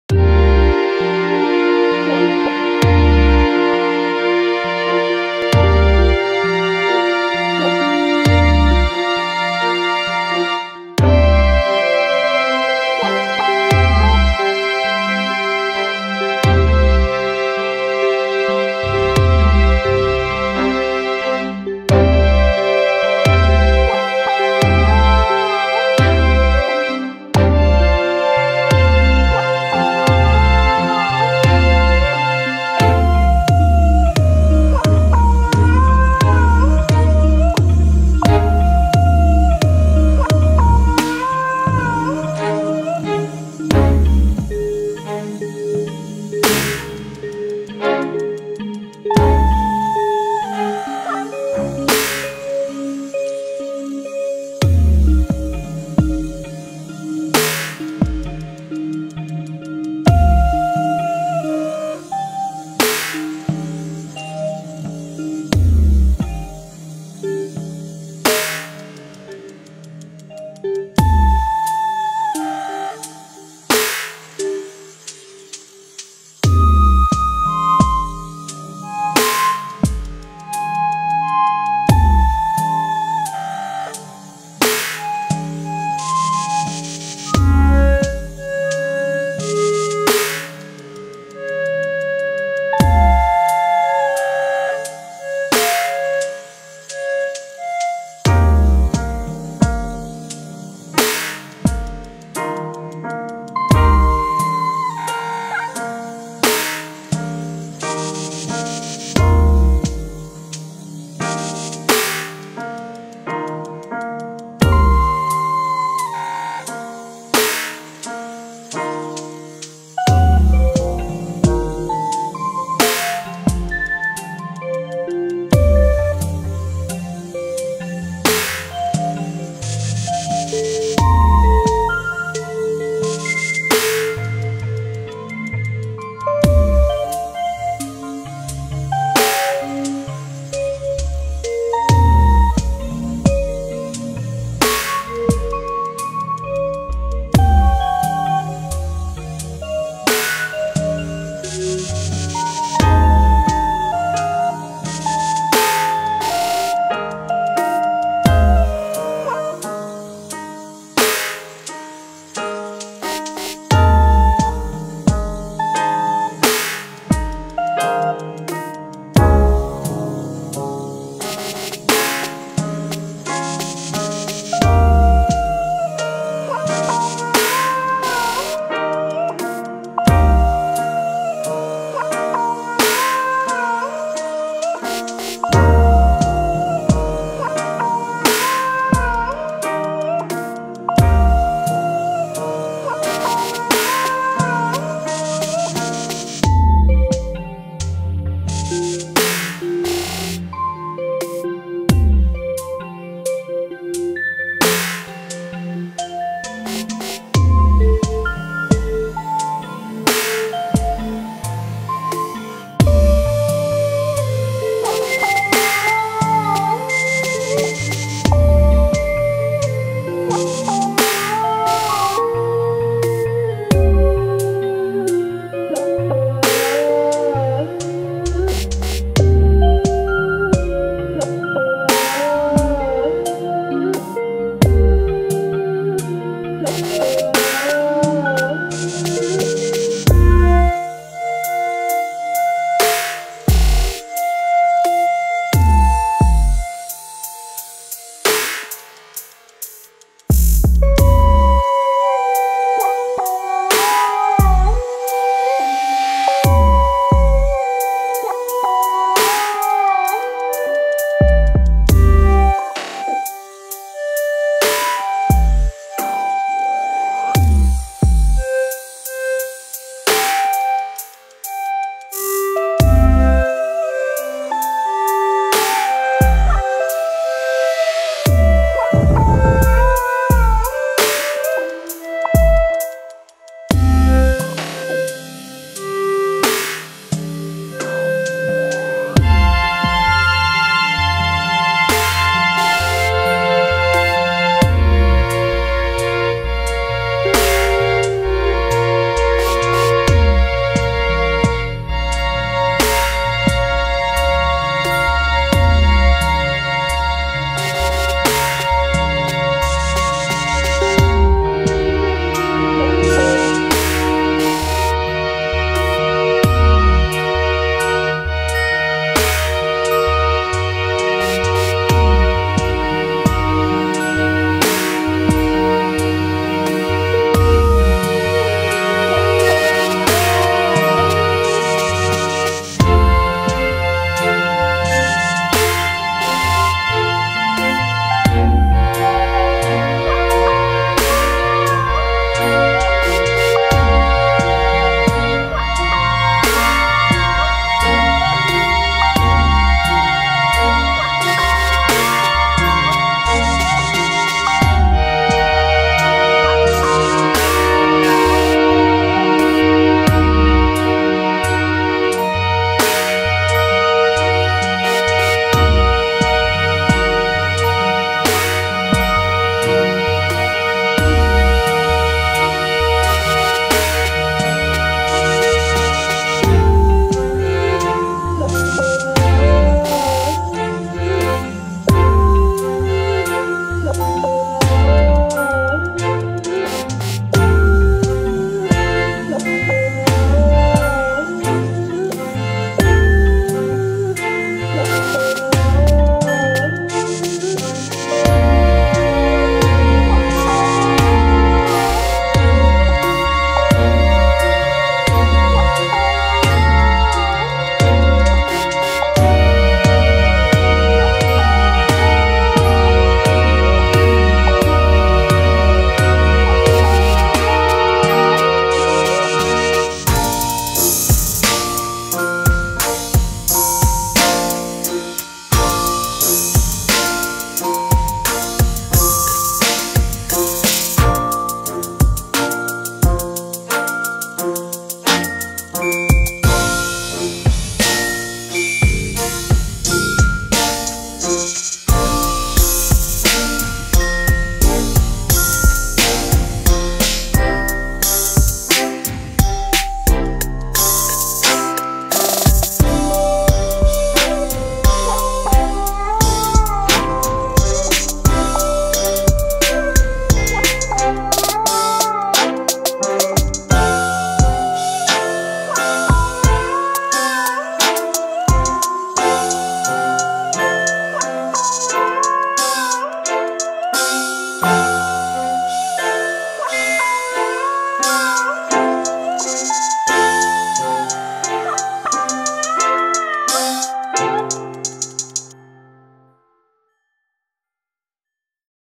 p0p02
Starting with a joyful introduction, this song delivers an evolving and contrastingsoundscape that includes emotions from all ends of the spectrum from happiness to curiosity to darker themes like sorrow, followed by recaps in structure and returning to contentment. Halfway one might find themselves asking where else can this song take me? Then the song delivers again and again until the final sequence brings the listener back into the place they’re at, begging for another listen.